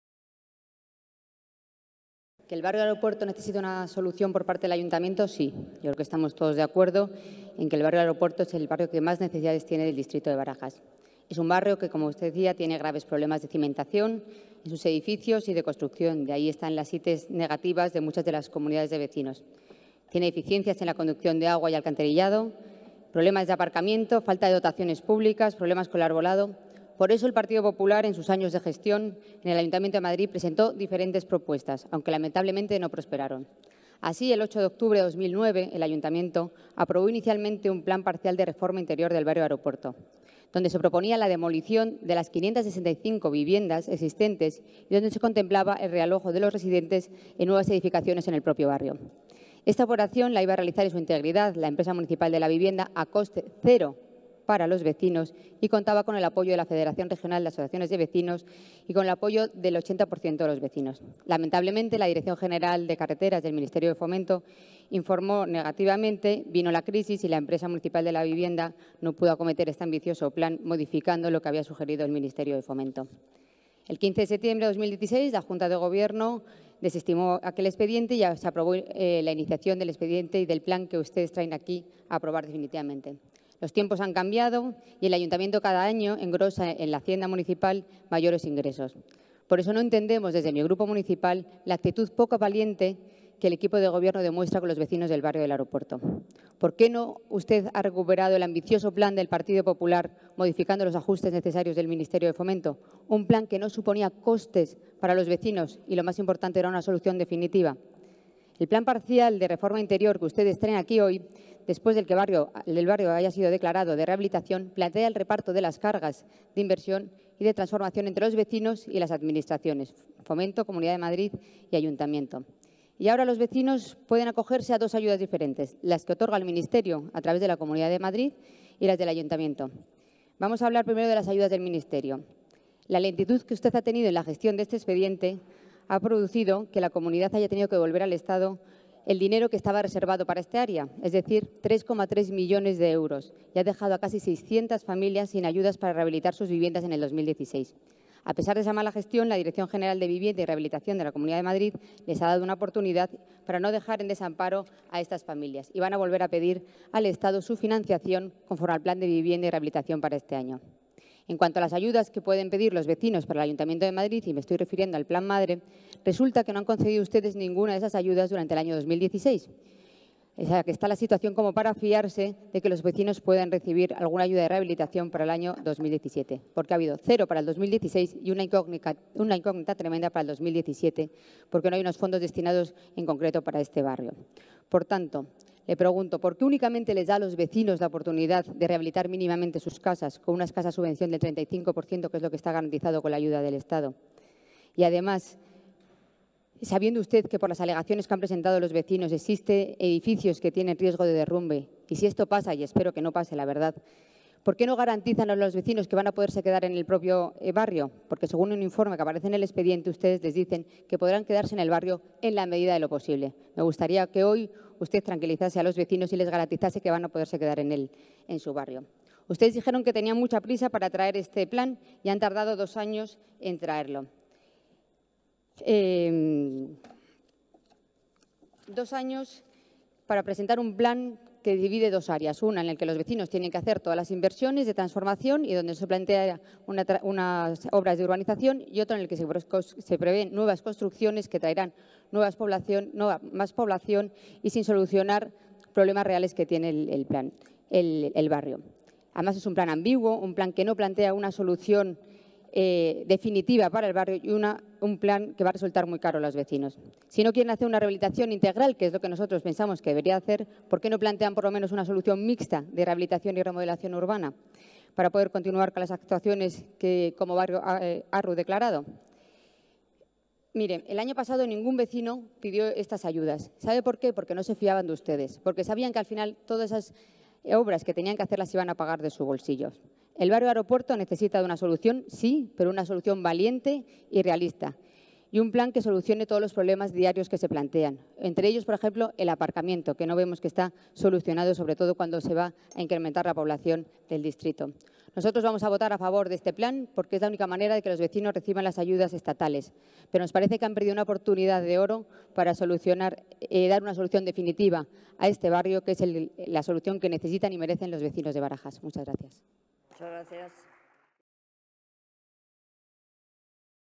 Maíllo pide a Calvo celeridad y que tranquilice a los vecinos del Barrio del Aeropuerto
Intervención de Almudena Maíllo en el Pleno Ordinario del Ayuntamiento de Madrid de 26 de abril de 2017.

Punto 22.- Propuesta del Área de Gobierno de Desarrollo Urbano Sostenible para aprobar definitivamente, con estimación parcial de las alegaciones presentadas, el Plan Parcial de Reforma Interior del Área de Planeamiento Remitido 21.02 “Barrio del Aeropuerto”. Distrito de Barajas.